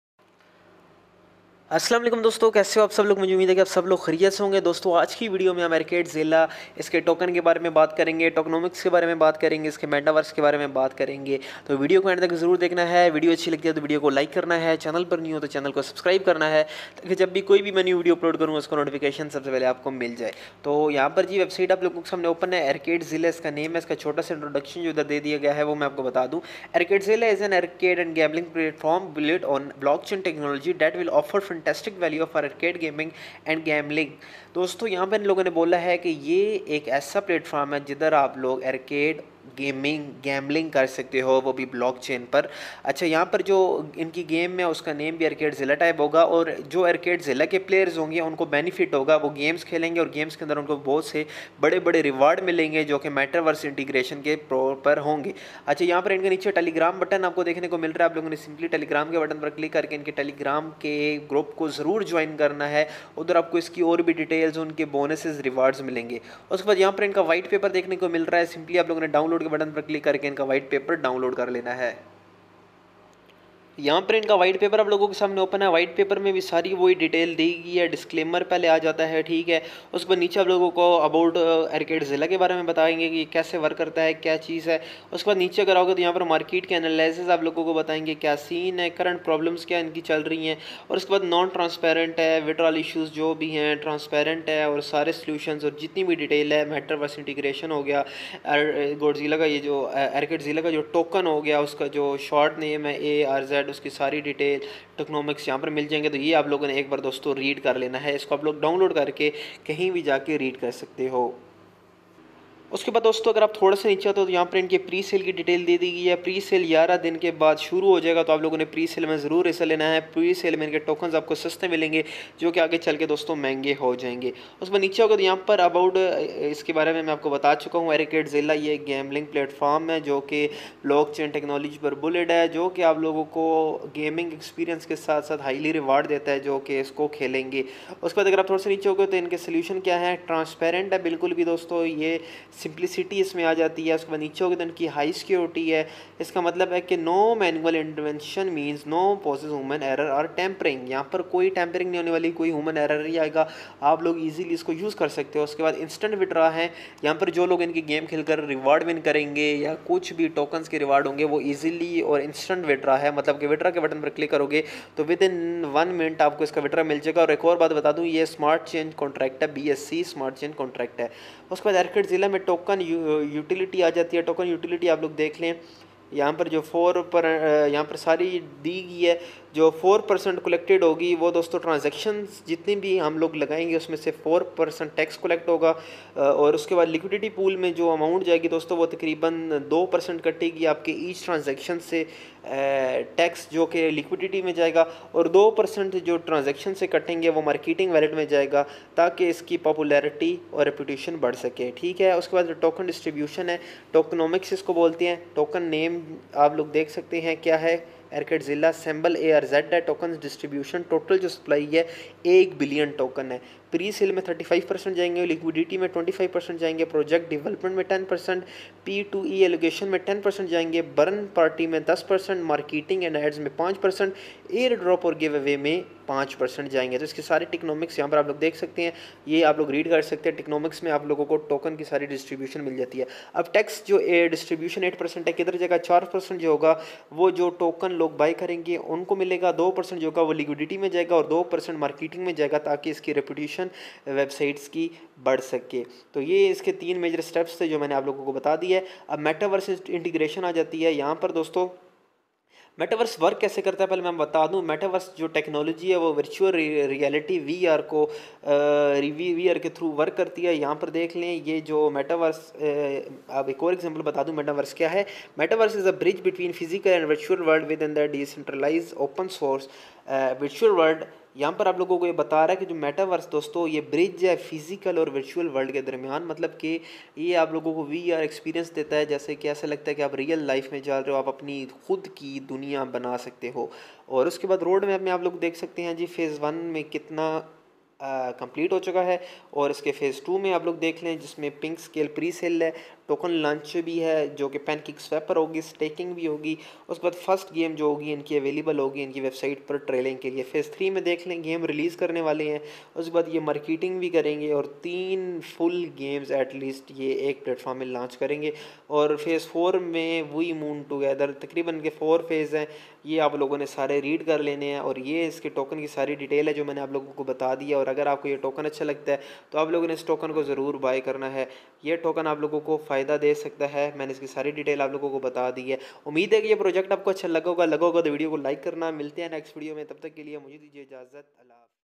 ArcadeZilla Project Full Review in Hindi | Don't Miss This Awesome Project
Hello Guys how are you in today video i will tell you about ArcadeZilla Project

The First Arcade & Gambling Plateform

Todays Video KeyWords :-

ArcadeZilla Review
ArcadeZilla Token Review
 Token Review

sometimes our view come from YouTube and other plateform like panel telegram and whatsapp groups